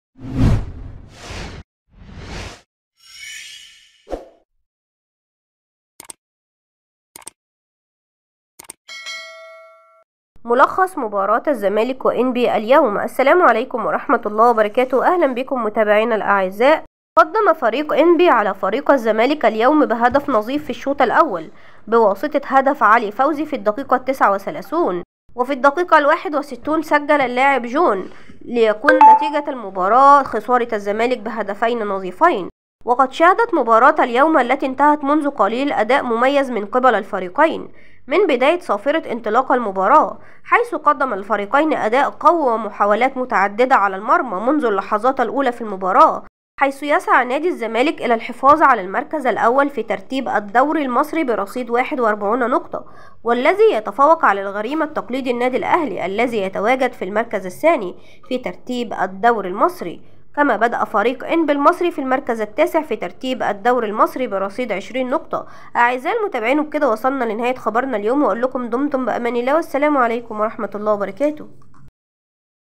ملخص مباراه الزمالك وانبي 2_0 في الدوري المصري تقدم انبي علي الزمالك الحكم يلغي الهدف الثالث لانبي
تمّ إيقاف "إعادة تشغيل المحادثة" في هذا العرض الأول.

ملخص واهداف مباراة الزمالك وانبي اليوم 2-0/تقدم انبي بهدفين فى شباك الزمالك